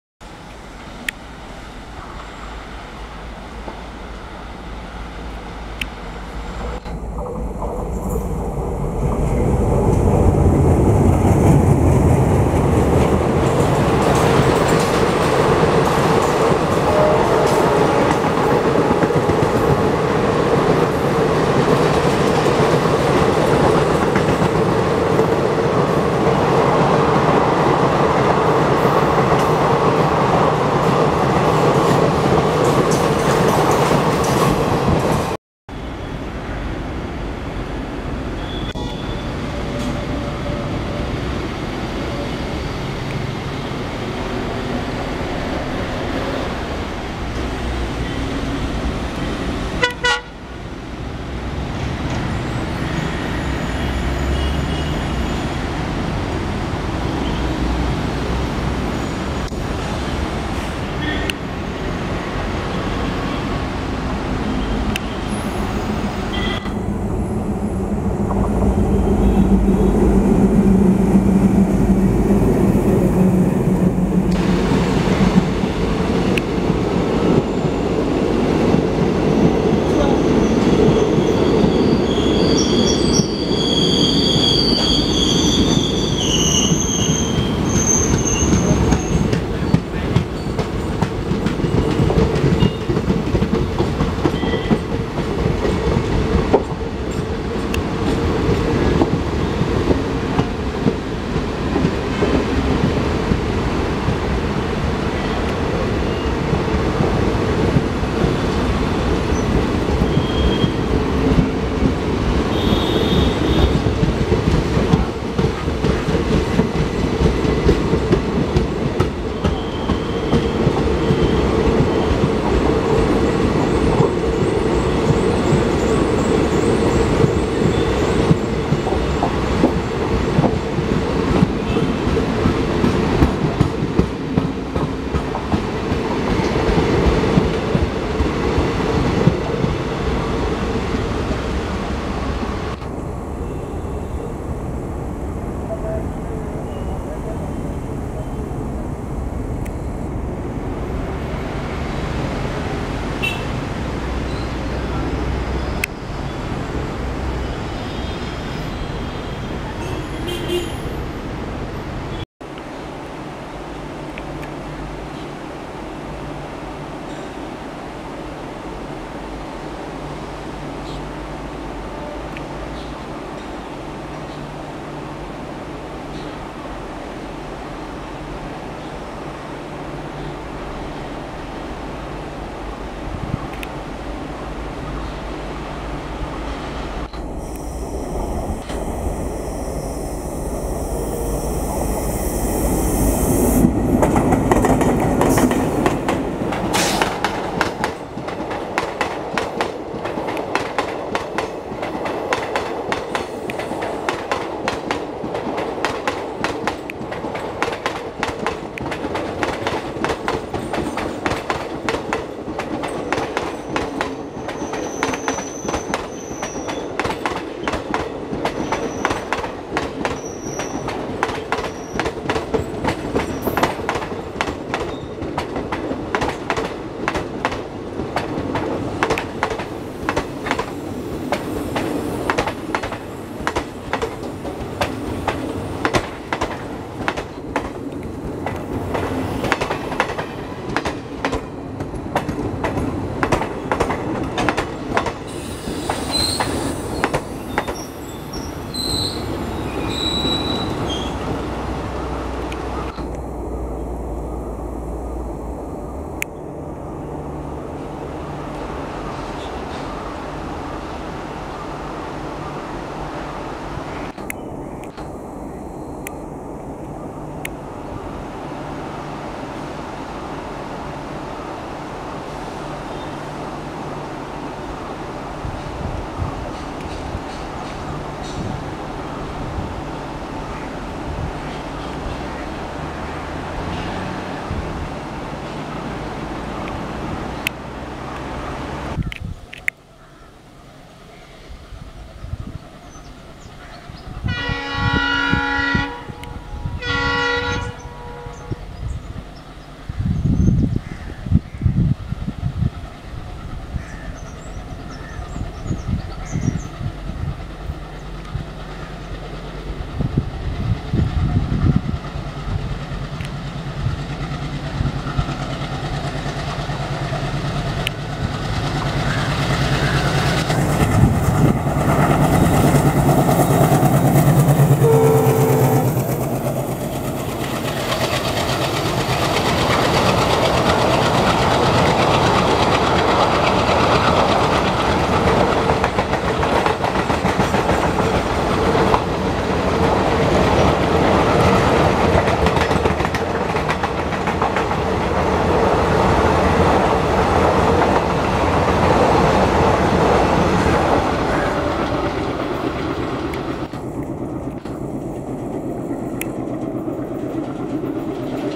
4 in 1 Compilation | Venad Express Compilation | വേണാട് എക്സ്പ്രസ്സ്‌ Southern Railways
INDIAN RAILWAYS

Ernakulam WDM-3A 14075

16301/Venad Express
വേണാട് എക്സ്പ്രസ്സ്‌     वेणाड़ एक्सप्रेस
SRR/Shoranur Junction -- TVC/Thiruvananthapuram Central (Trivandrum)